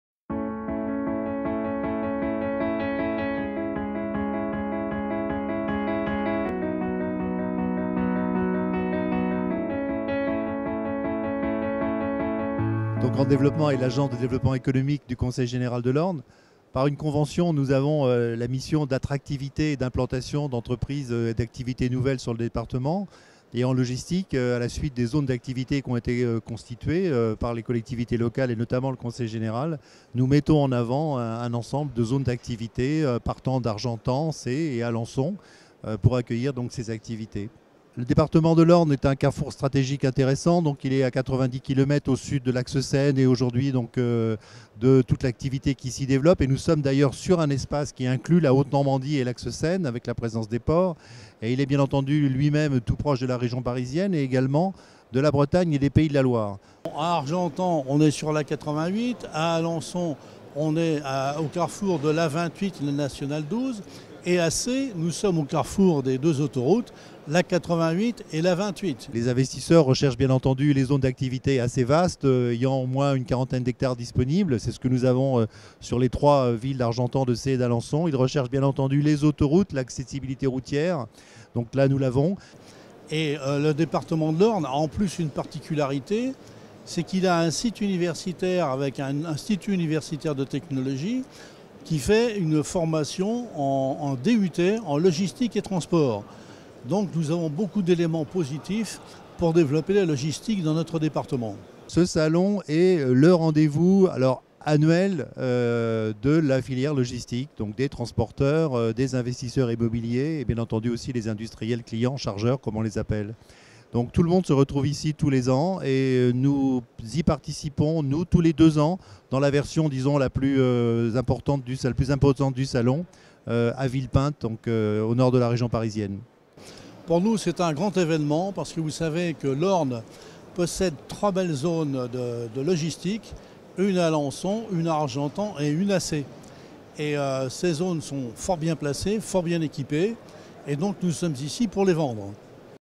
Orne Développement au STIL 2014 - Normandie - Logistique
Orne Développement, Agence de développement économique du Conseil général de l'Orne, participait à la Semaine Internationale du Transport et de la Logistique (SITL) à Paris Villepinte du 1er au 4 avril 2014 sur le stand collectif normand de 300 m² auprès de Logistique Seine Normandie (LSN), la Fédération des Clubs Logistique de Basse-Normandie (FCL BN) et d'une trentaine d'entreprises et acteurs de la logistique normande.
Une façon concrète de promouvoir l'excellence logistique de la région et les disponibilités foncières et immobilière du territoire ornais auprès de quelques 40 000 visiteurs, 800 exposants et 80 pays.